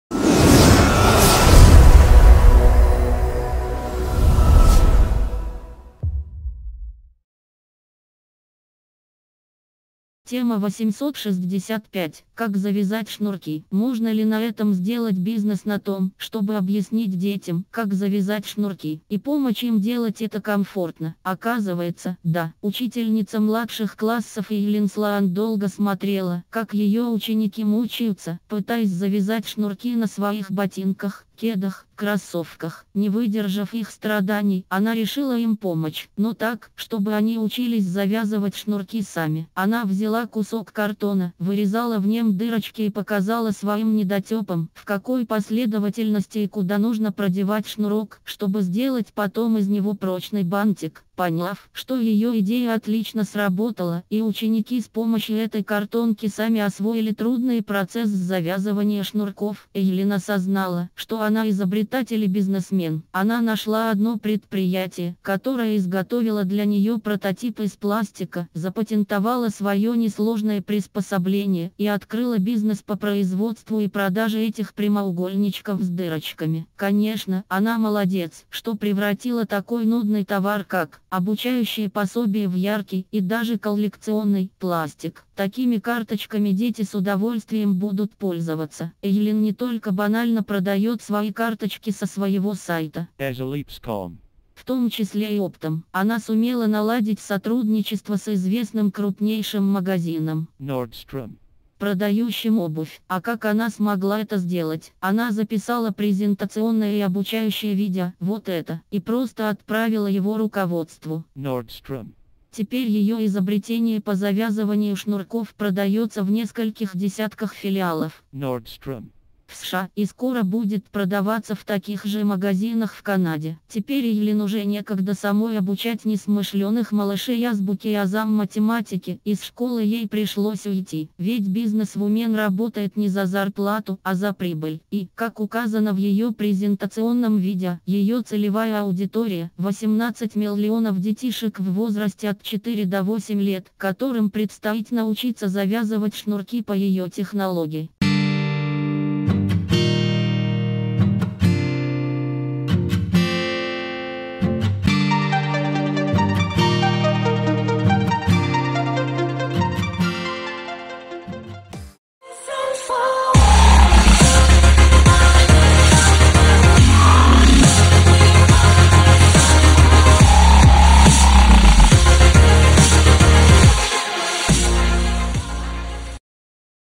Как завязать шнурки
Можно ли на этом сделать бизнес на том, чтобы объяснить детям, как завязать шнурки? И помочь им делать это комфортно. Оказывается, да.